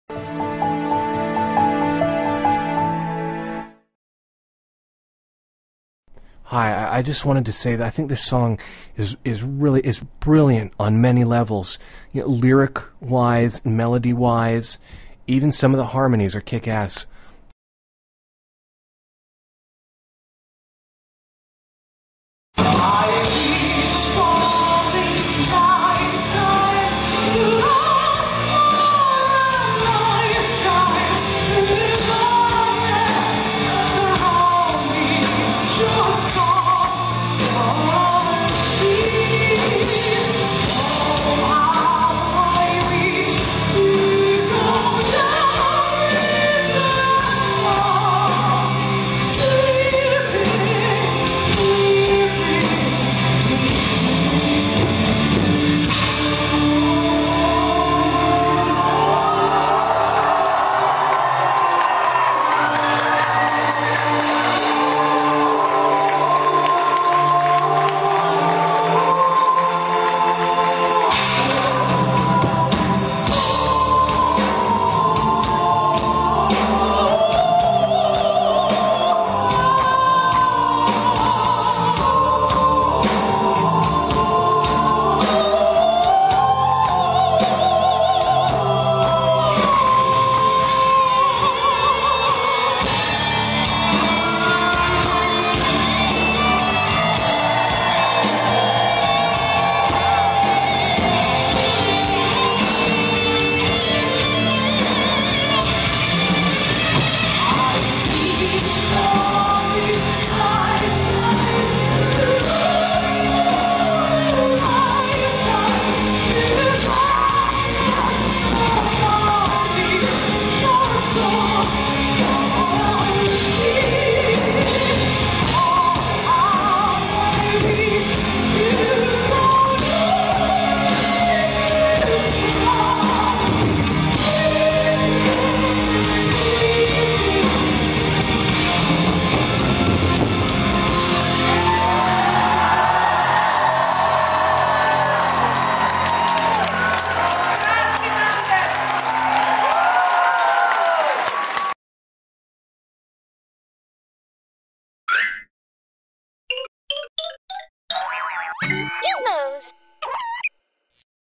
Tarja and Tuomas Bologna
Tuomas introduces the concert of Tarja @ Bologna